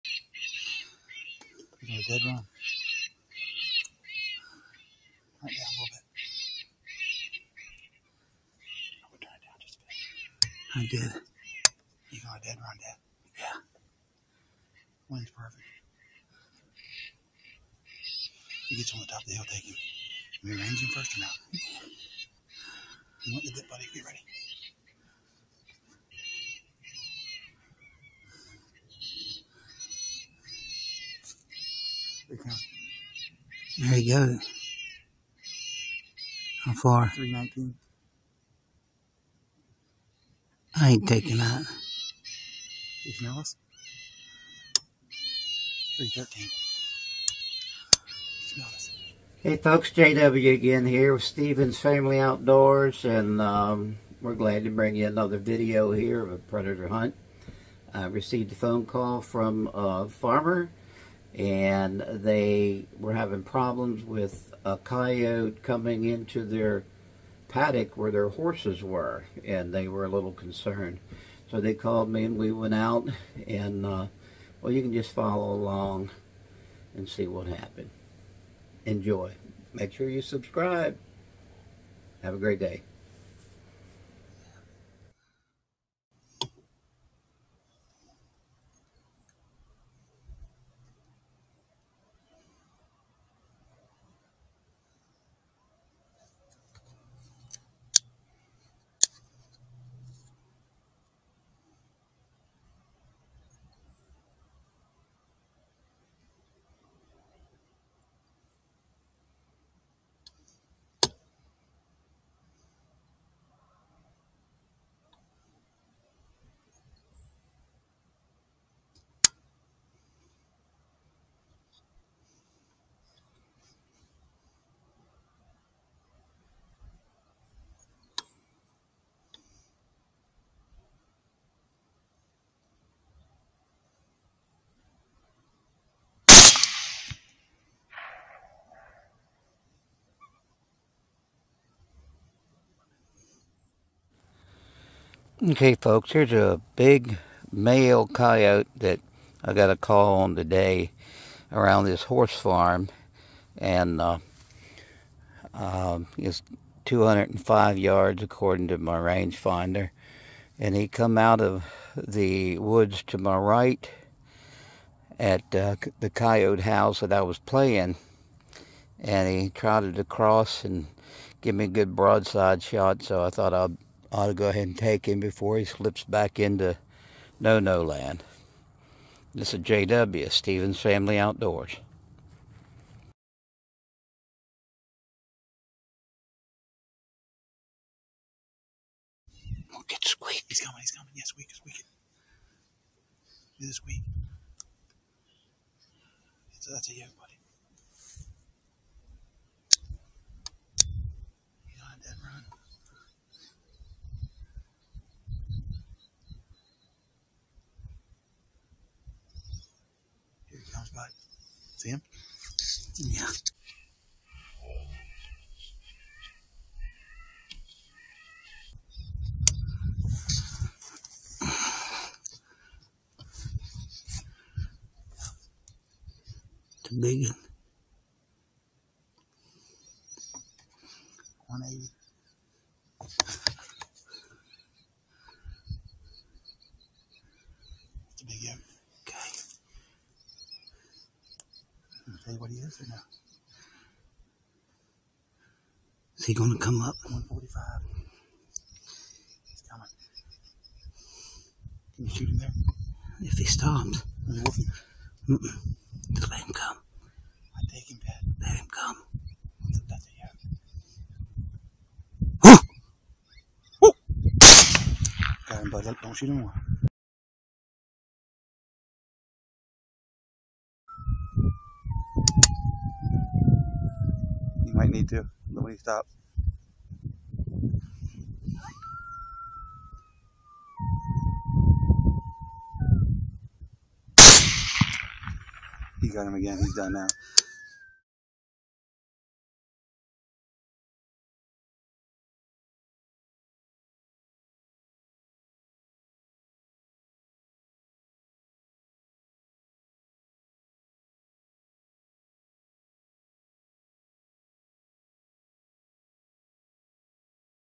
Coyote Control on a Horse Farm Predator Hunting
JW of Stevens Family Outdoors is at it again. Resolving the conflict between humans and animals. Predator hunting.